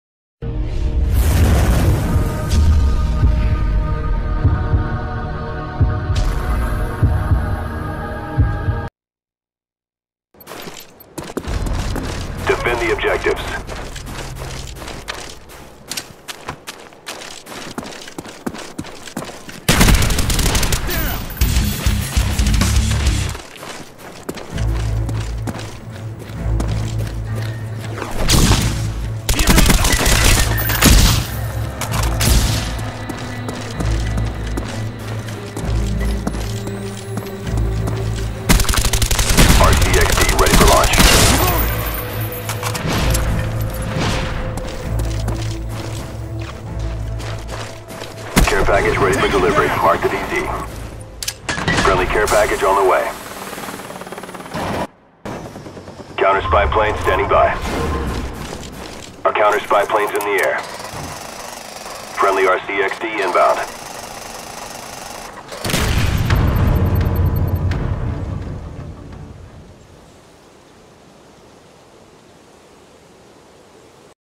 S&D Last Round
last round of s&d took out the 6 of them  18k 1D